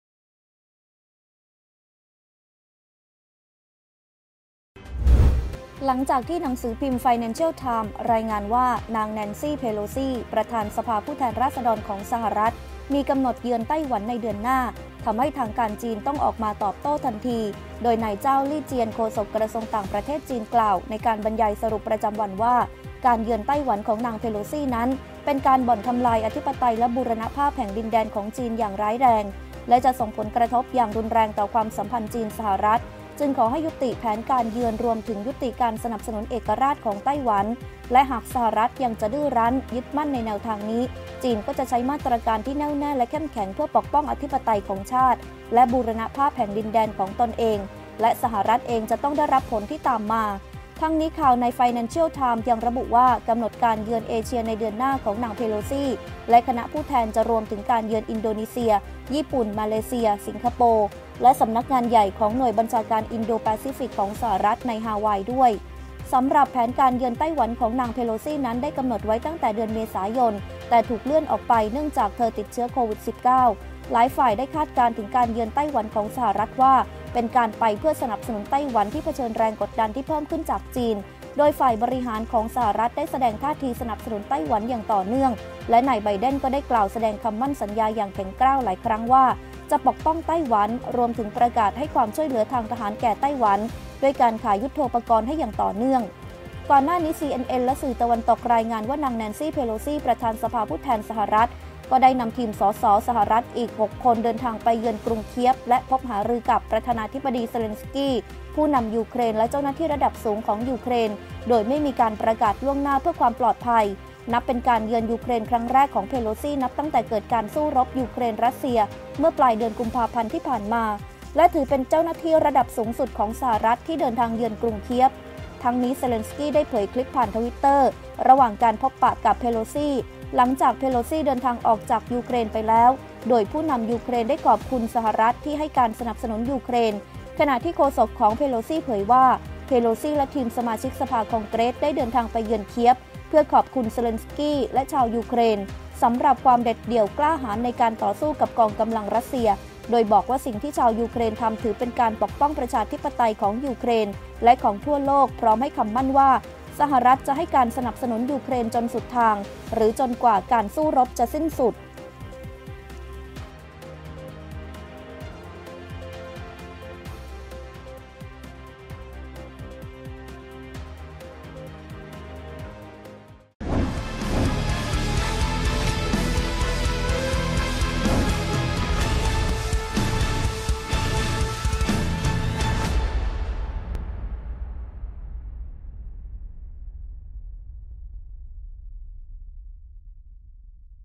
“จีน”ขู่สหรัฐให้“แนนซี่ เพโลซี่”เลิกเยือนไต้หวัน ลั่น ตอบโต้หนักแน่ | ข่าวด่วน | TOP NEWS
หลังจากที่หนังสือพิมพ์ไฟแนนเชียลไทมส์ รายงานว่า นางแนนซี่ เพโลซี่ ประธานสภาผู้แทนราษฏรของสหรัฐ มีกำหนดเยือนไต้หวันในเดือนหน้า ทำให้ทางการจีนต้องออกมาตอบโต้ทันที